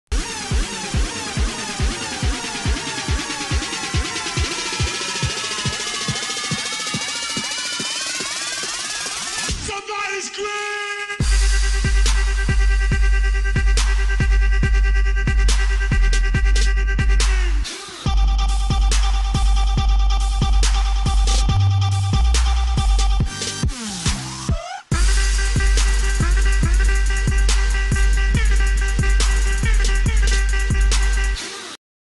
Polis'den dayak yiyen sapık siyasetci - Gif
Kendisini milyonların izledigi bir siyasetci olan  chp genel başkanının agza alınmıyacak kelimeleri sarfettigin'den dolayı şiddetle kınıyor bu gifi ona layık görüyoruz.